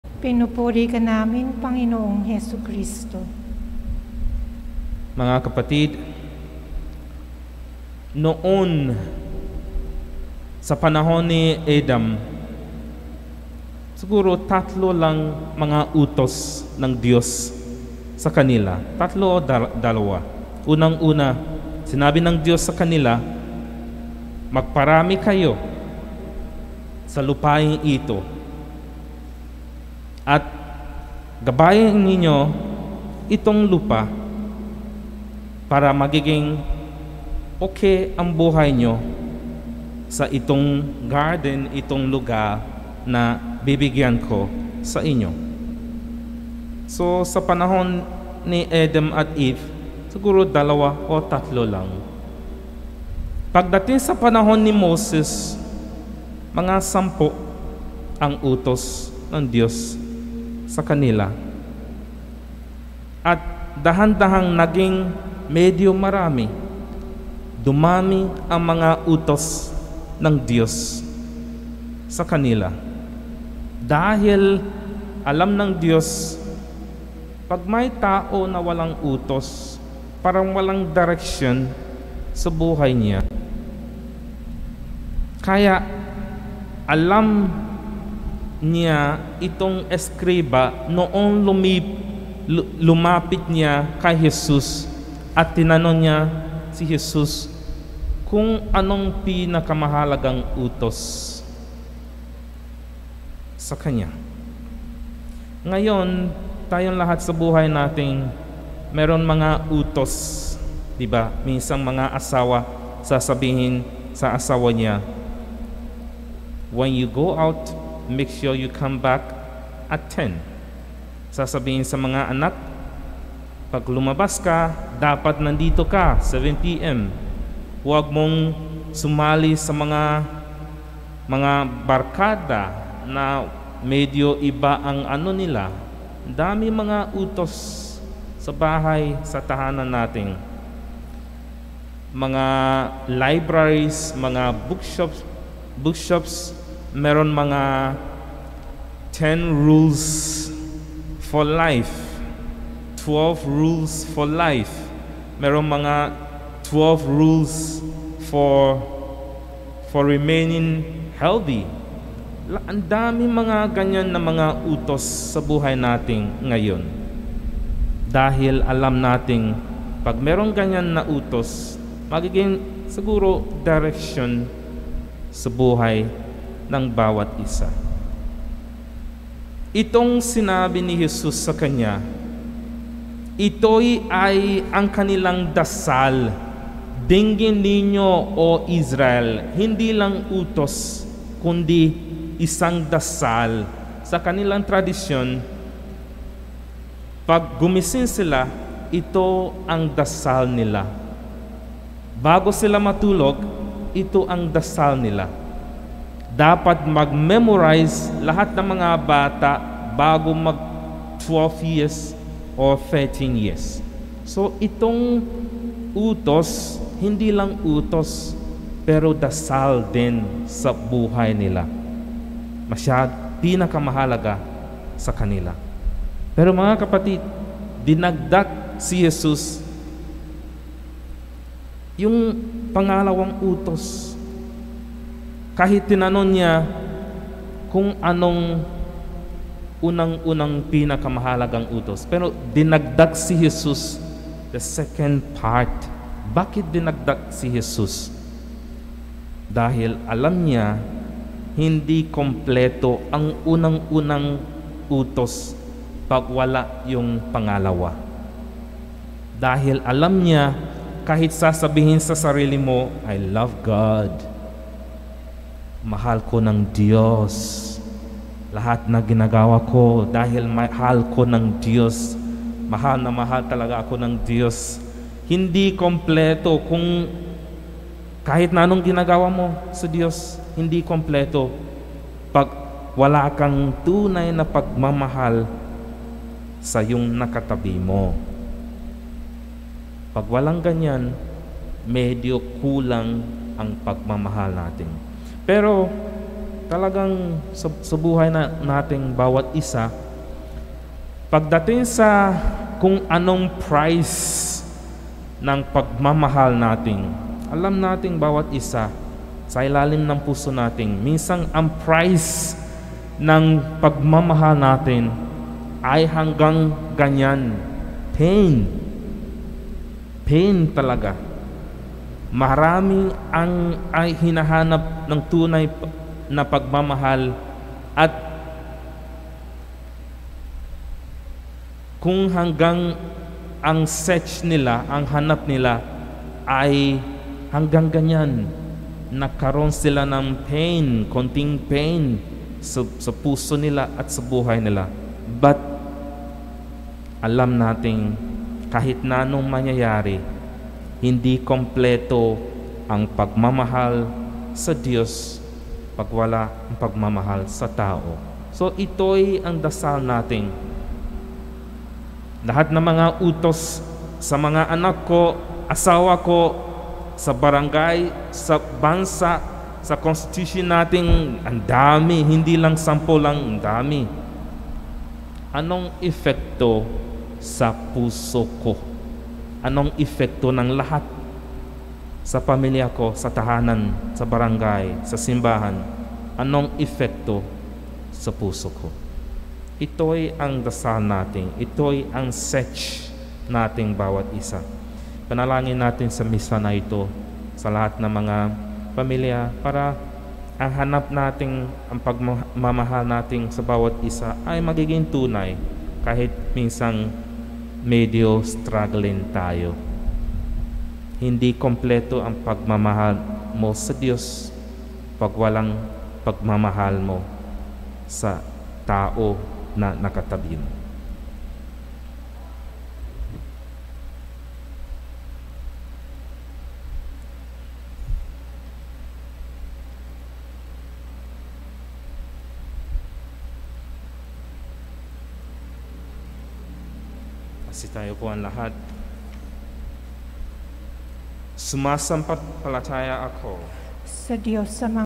Homily By Fr. Andrew Amanor, SVD - October 30 2021 - 31st Sunday in Ordinary Time
Live 6:00 PM  Anticipated Sunday Holy Mass - October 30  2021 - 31st Sunday in Ordinary Time

Online Mass Schedules -- Weekday (Daily) Mass - 6:30 AM & 6 PM; Sunday Masses - 8AM, 10 AM & 4PM